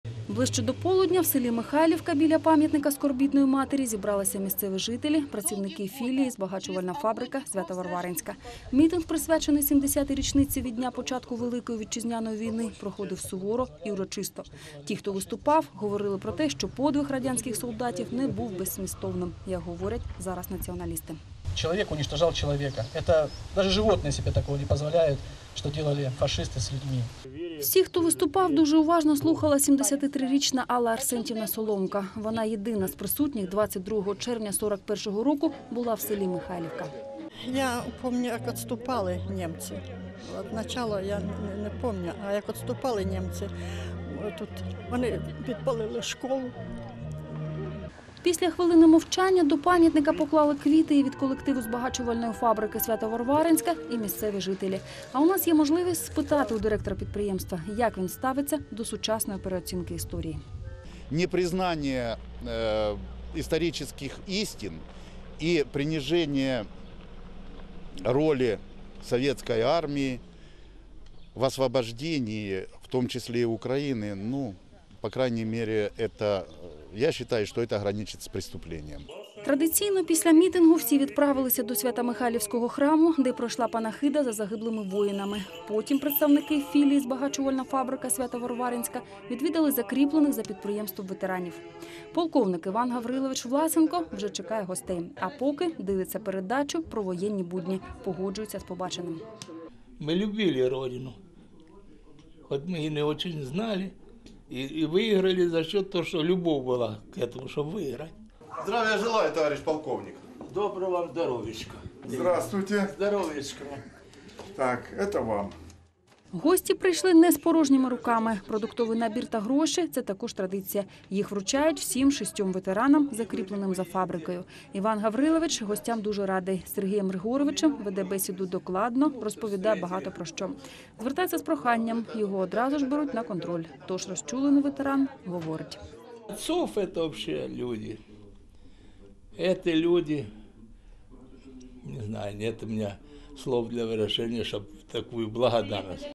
22.06.11 В селе Михайловка чествовали ветеранов
Торжественный митинг в День скорби о павших за годы ВОВ прошел и в селе Михайловка Красноармейского района. Не забыли и о ветеранах. В данный момент в населнном пукте в живых осталось лишь шестеро человек, прошедших войну.